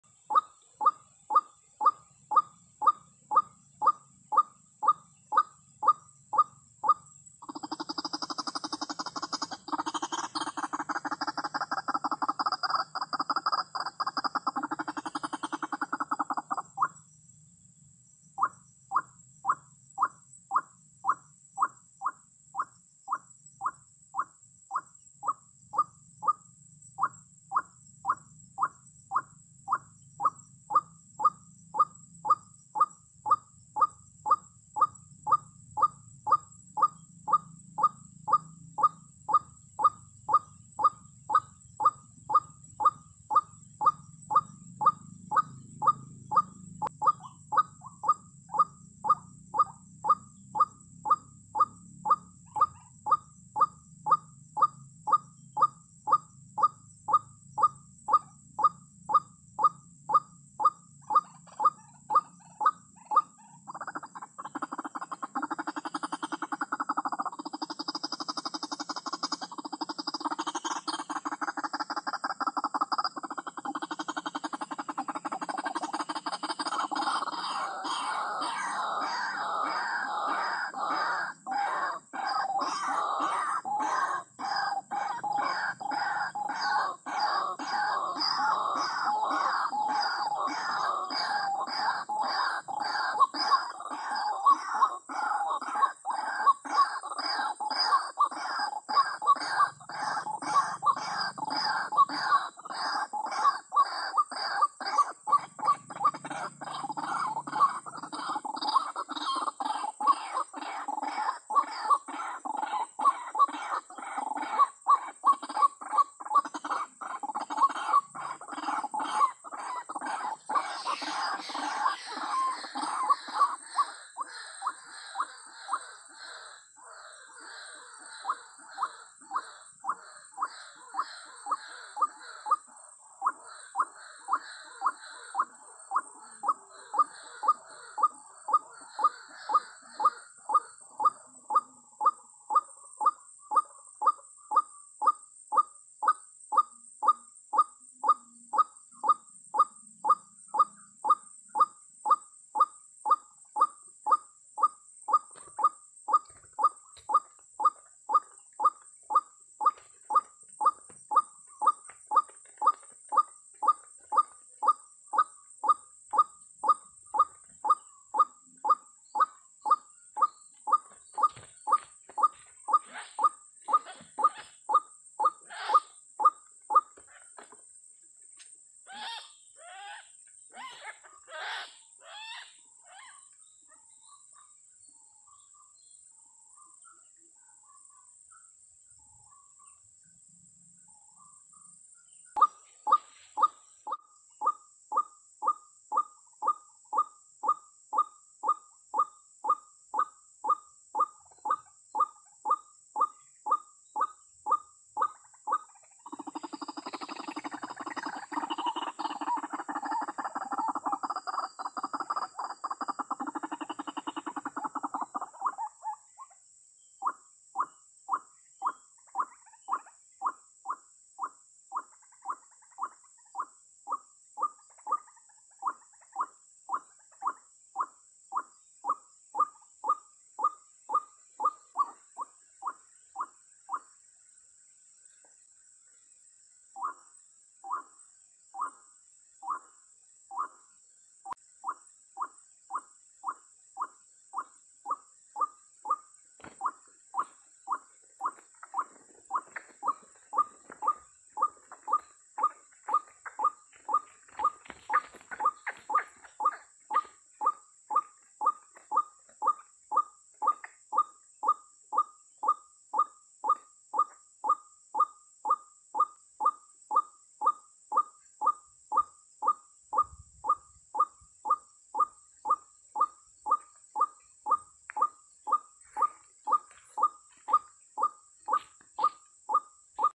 🔴jerat burung ruak ruak di lokasi Pasiran teryata ruak⁉️⁉️ruakruak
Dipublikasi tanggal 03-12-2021
🔴jerat burung ruak ruak di lokasi Pasiran teryata ruak⁉️⁉️

Jangan lupa bersyukur dan berdoa semoga terhibur.